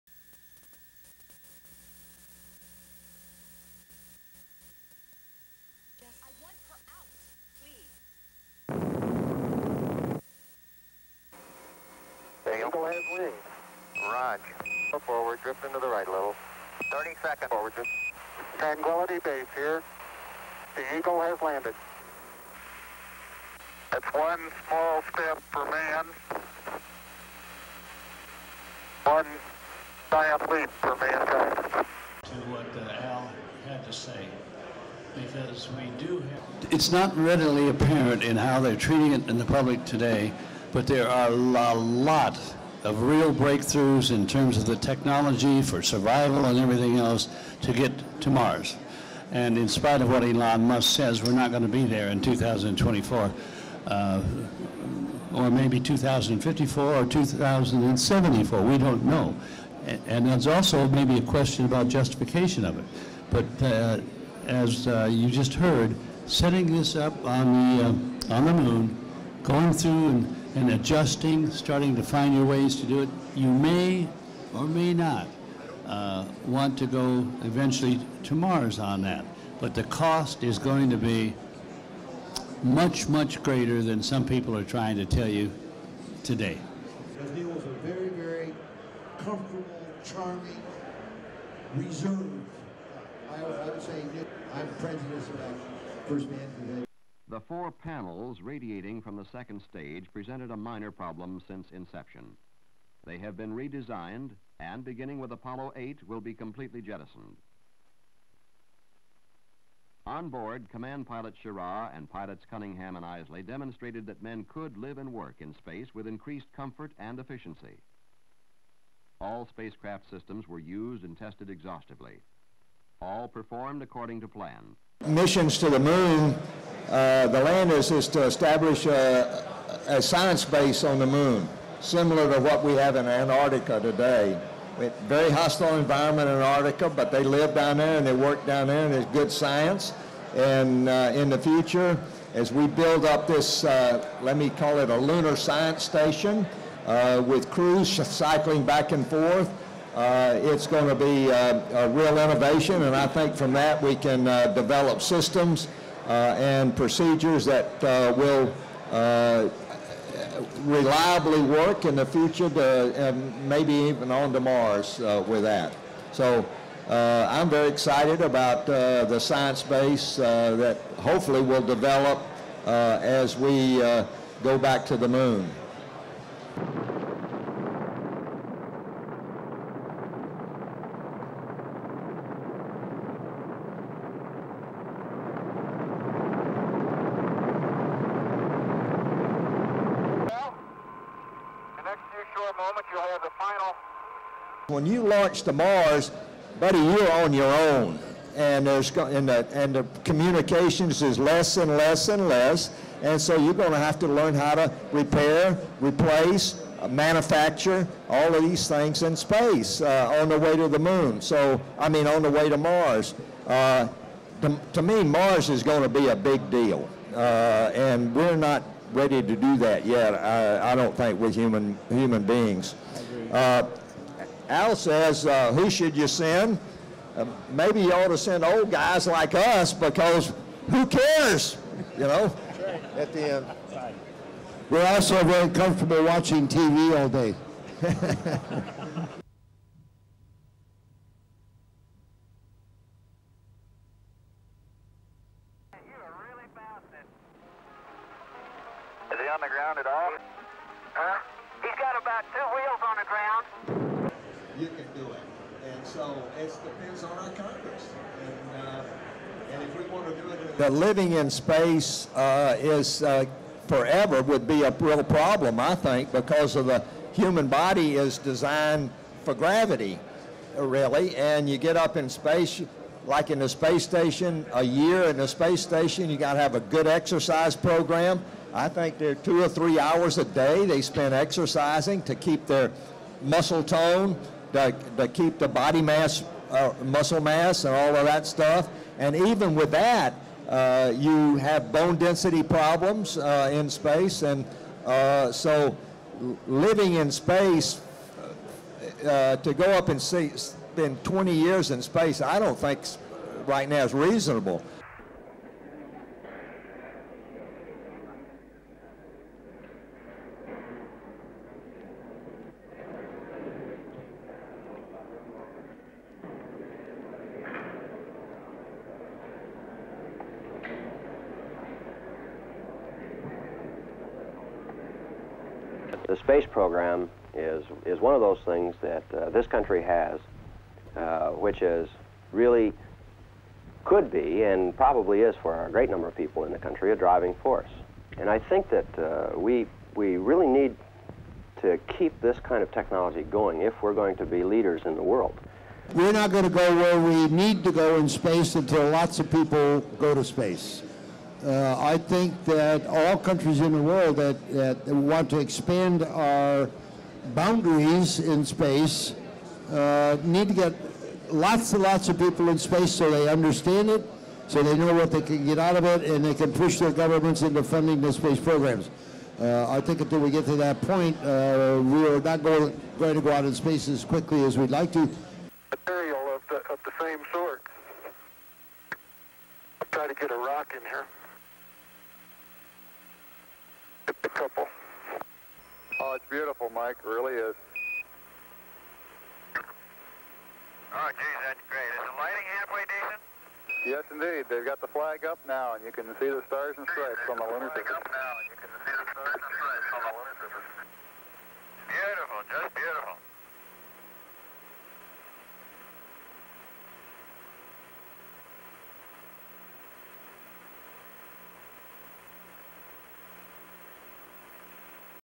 US Apollo astronauts say life on Mars will be tough
(20 Jun 2019) LEADIN:
Three astronauts key to the early US space programme, say it'll be some time before we head to Mars.
The former astronauts, who took part in a number of the Apollo missions, are also hopeful that one day science missions will be based on the moon.
The comments came in the lead up to the 50th anniversary of man landing on the moon in 1969.
STORYLINE:
The Apollo 11 astronauts: Neil Armstrong, Michael Collins and Edwin "Buzz" Aldrin prepare for launch.
On July 20, 1969, at 9:32 a.m. EDT, a Saturn V rocket lifted off from launch complex 39A at NASA's Kennedy Space Centre in Florida.
About 12 minutes later, the Apollo 11 crew was in orbit.
Neil Armstrong made history books with his words, "It's one small step for man, one giant leap for mankind."
In a joint public appearance, the US astronauts involved in groundbreaking US space programs took questions at this week's Paris Air Show.
Walter Cunningham, 87, was part of the Apollo 7 mission in 1968.
It was the first mission in the Apollo program to carry a crew into a low earth orbit.
It was also the first US spaceflight to carry astronauts since the flight of Gemini XII, known as Apollo 1, in November 1966. That mission was designed to test the Apollo command and service module (CSM). But it never made it.
During a launch rehearsal at Cape Kennedy the cabin caught fire killing all three crew members.
In the 21st century, there's now talk of sending a misison to Mars.
Cunningham says, "there are a lot of real breakthroughs in terms of the technology for survival and everything else to get to Mars. And despite Elon Musk says, we're not going to be there in 2024, or maybe 2054 or 2074, we don't know."
But to go to Mars, "the cost is going to be much, much greater than some people are trying to tell you today. "
Lunar Module Pilot, Charlie Duke, 83, has walked on the moon.
Apollo 16 was the tenth mission to land on the moon and was crewed by Commander John Young, and Command Module Pilot Ken Mattingly.
Duke explains, "in the future, as we build up this, let me call it a lunar science station with crews cycling back and forth, it's going to be a real innovation and I think from that we can develop systems and procedures that will reliably work in the future, maybe even go on to Mars with that. So, I'm very excited about the science base that hopefully will develop as we go back to the moon."
Duke was the tenth and youngest person to walk on the moon aged 36.
"When you launch to Mars buddy, you are on your own," he says.
"Communications is less and less and less and so we're going to have to learn how to repair, replace, manufacture all of these things on the way to the Moon, I mean on the way to Mars."
Even if communication and infrastructure were feasible on Mars, there is still a whole host of problems when it comes to living in space.
"Living in space is... forever would be a real problem I think because of the human body is designed for gravity really and you get up in space like in the space station a year, and a space station you've got to have a good exercise program. I think there two to three hours a day they spent exercising to keep their muscle tone, they keep the body mass, muscle mass and all of that stuff. And even with that, you have bone density problems in space and so... living in space, to go up and spend 20 years in space, I don't think right now is reasonable."explains Duke.
Alfred Worden was the Command Module Pilot for Apollo 15,  the ninth crewed Apollo mission that launched July 26, 1971.
Meanwhile, Worden in the command module reached a distance of 2,235 miles from his crewmates.